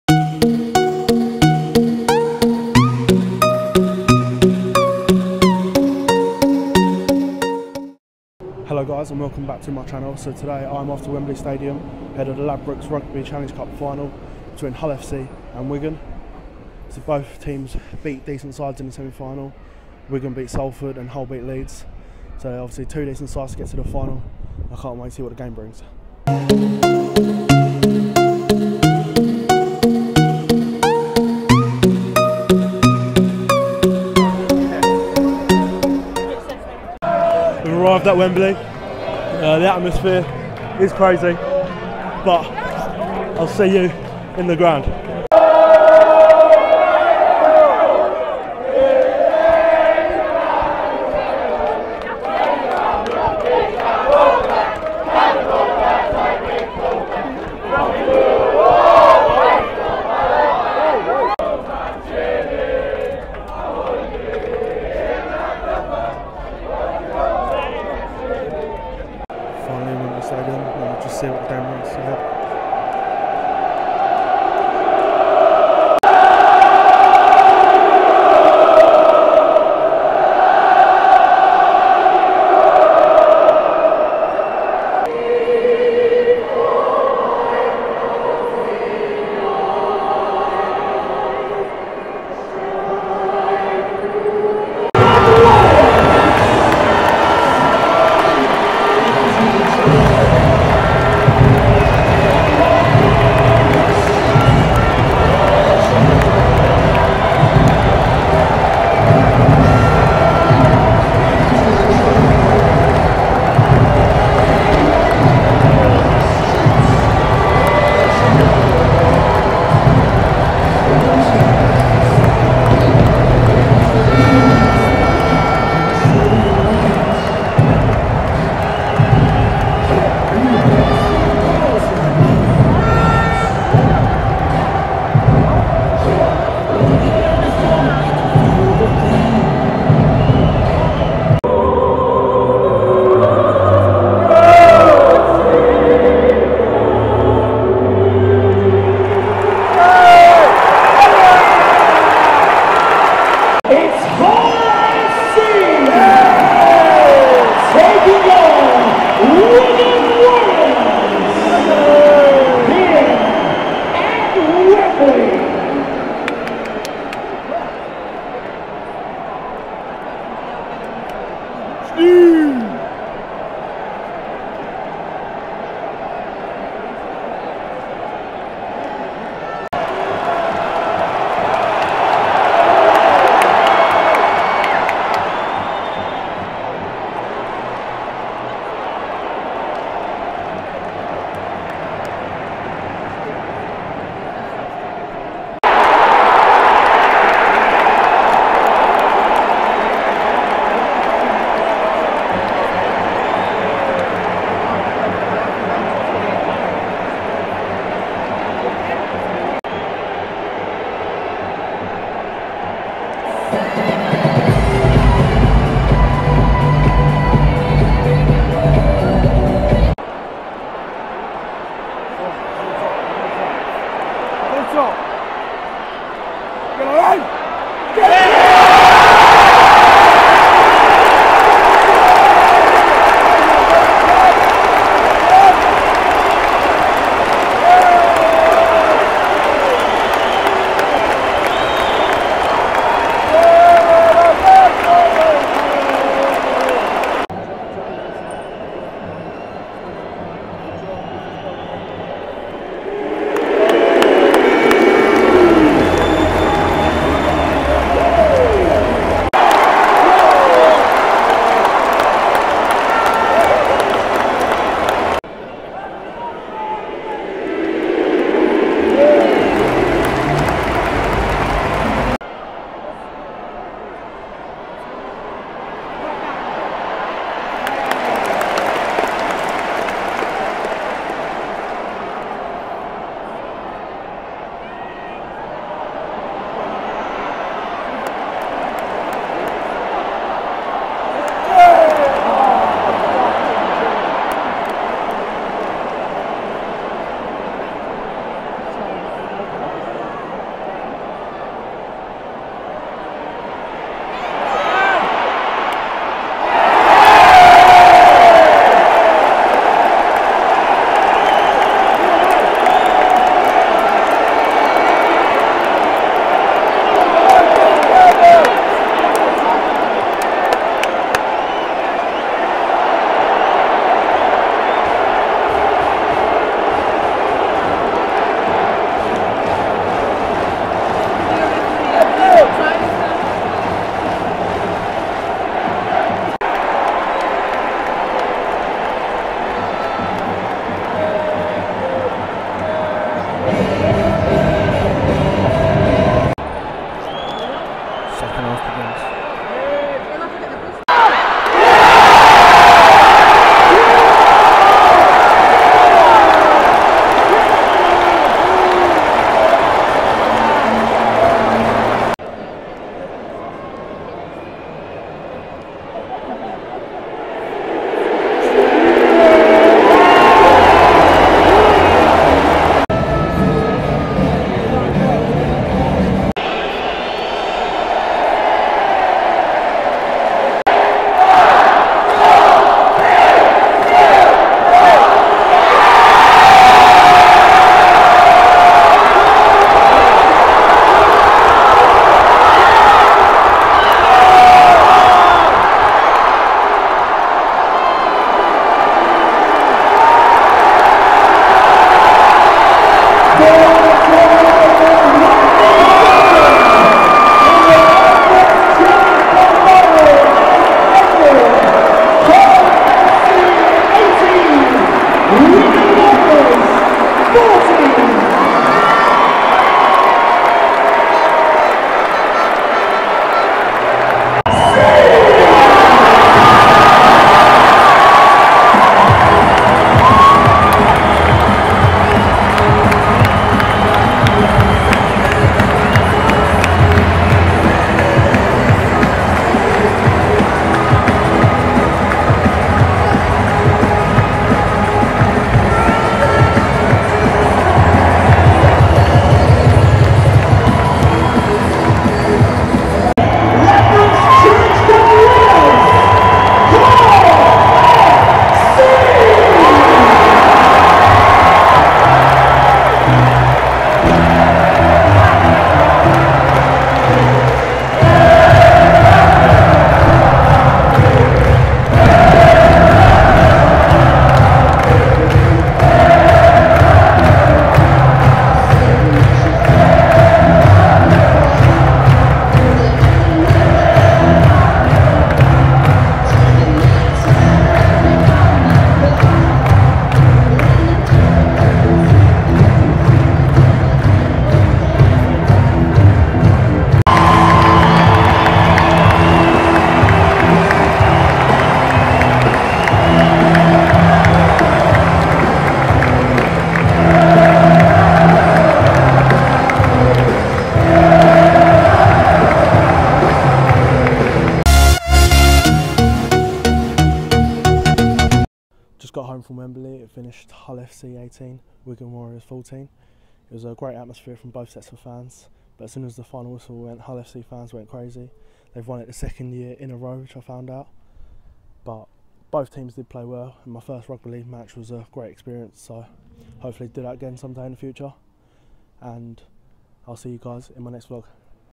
Ladbrokes Challenge Cup Final: Hull FC vs Wigan Warriors
My experience from the Ladbrokes Challenge Cup Final at Wembley Stadium